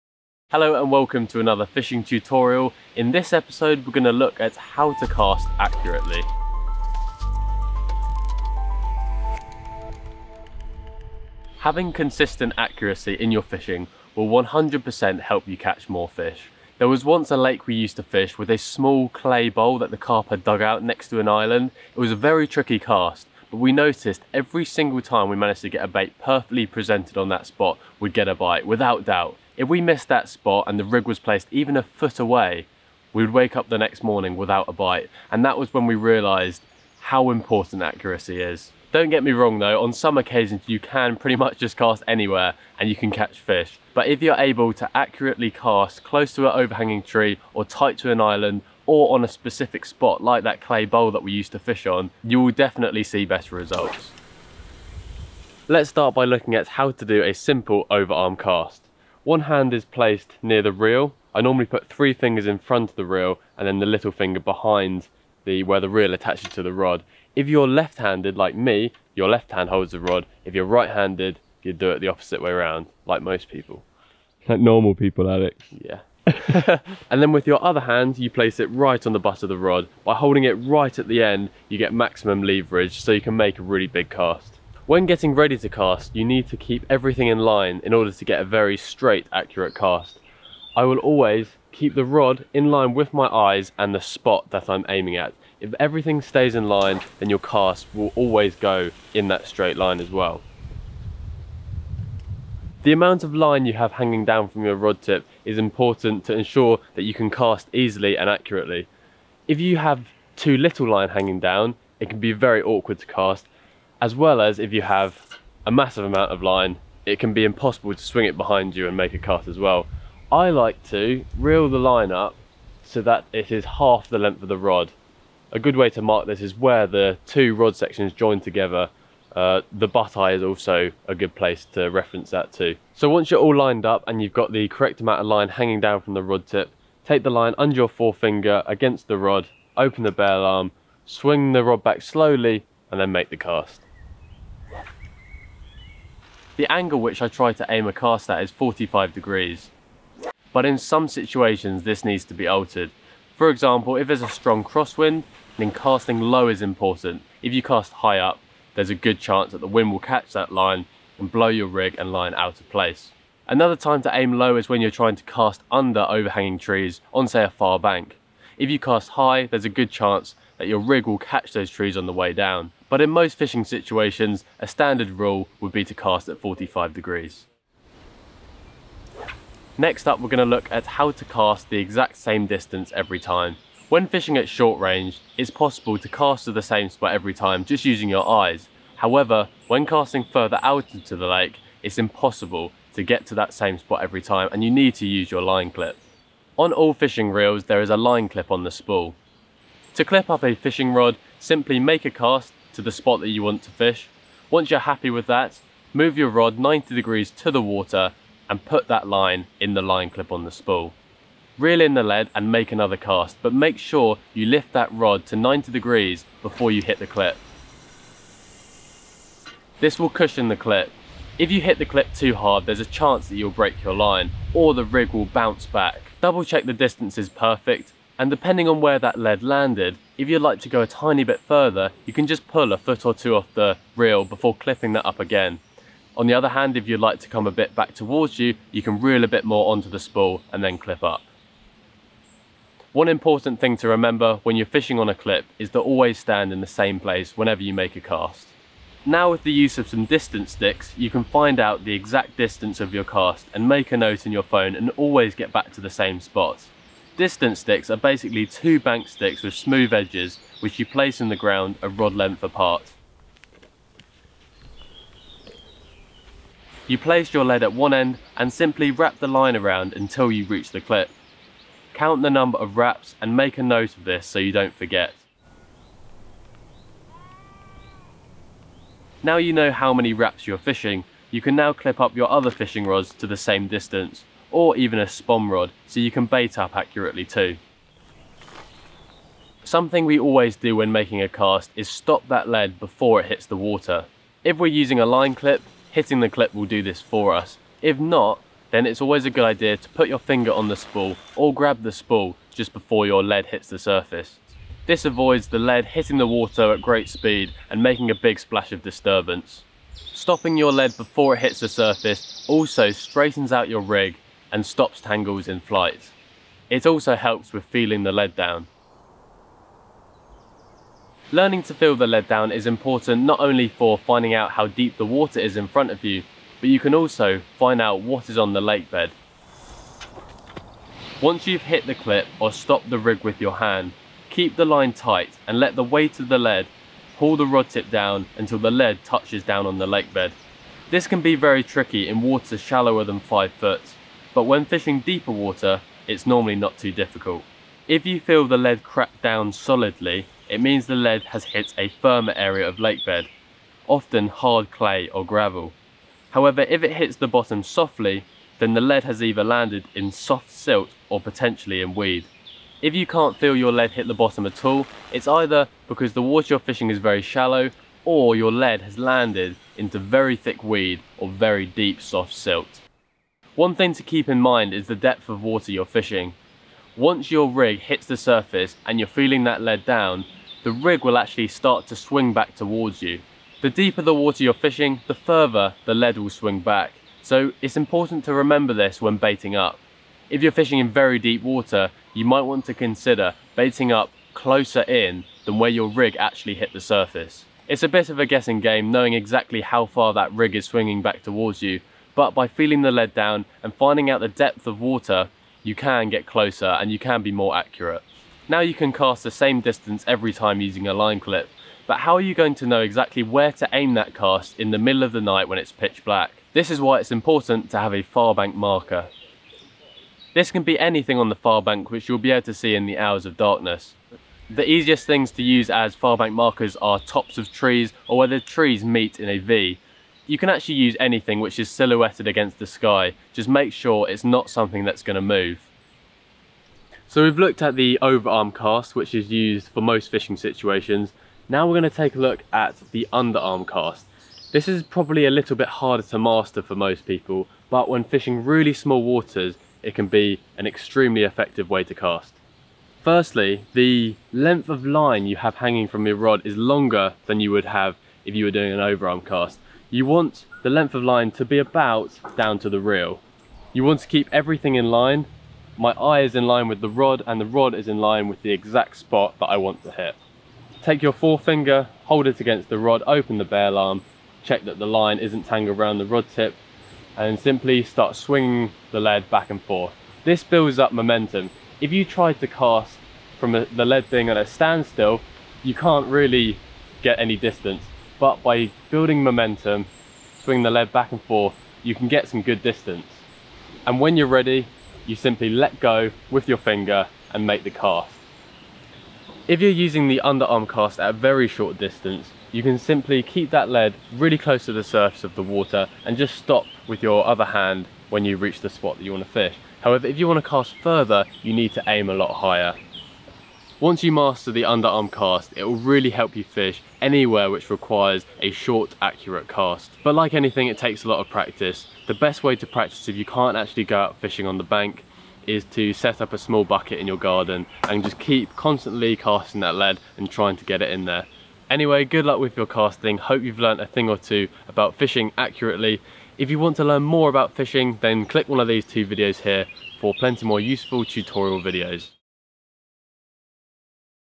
How To Cast More Accurately When Fishing
Casting accurately is key to success in any style of fishing. Watch to see how we cast to the same spot every time!

I created Fishing Tutorials to help people learn about fishing and catch more fish!